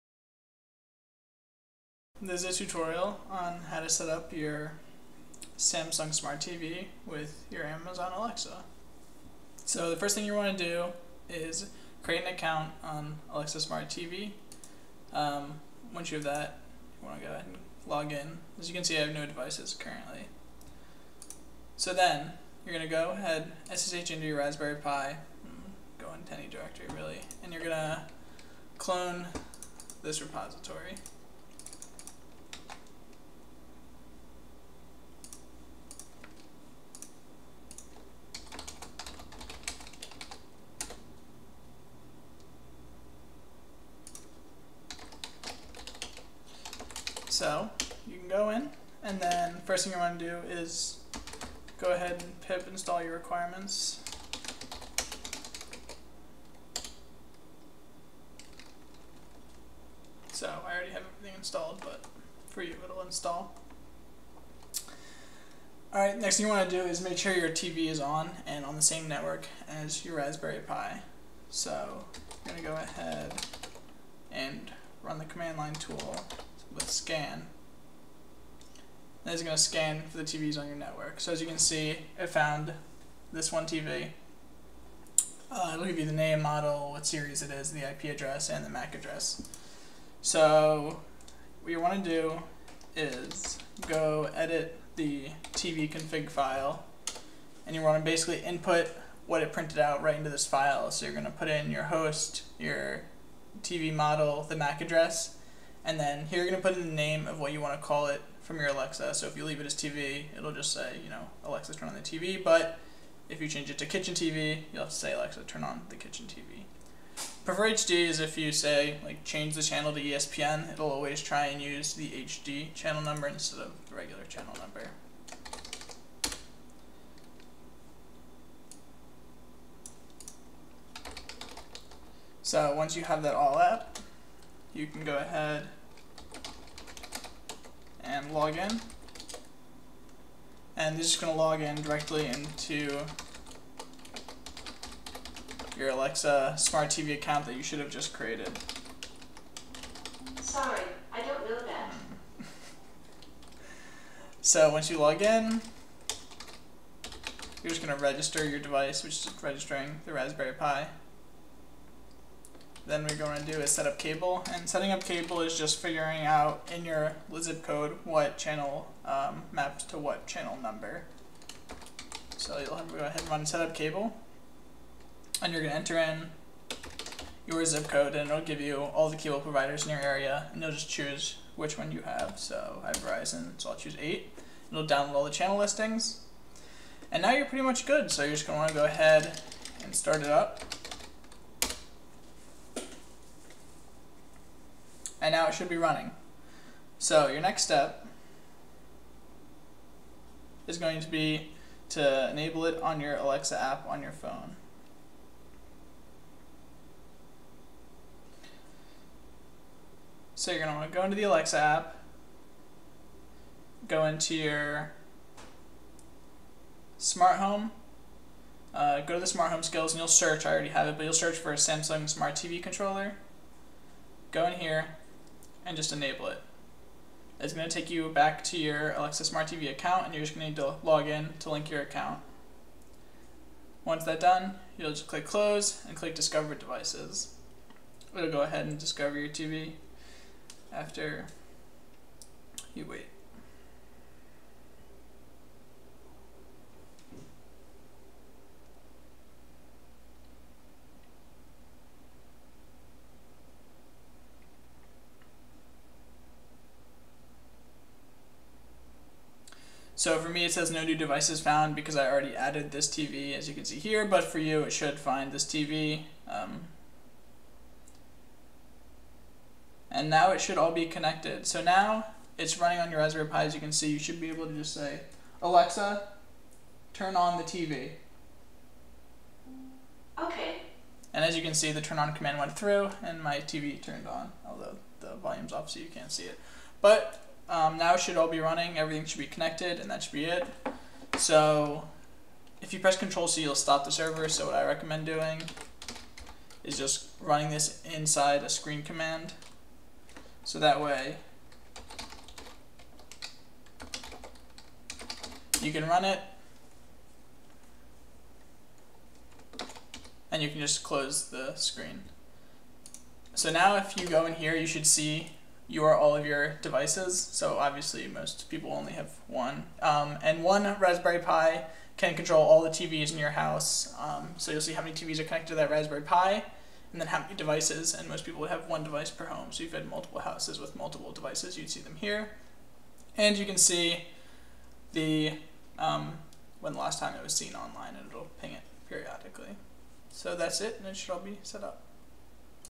Setup your Alexa to control your Samsung SmartTV
A tutorial to show you how to setup your Alexa to control your Samsung Smart TV. Requires you to have a RaspberryPi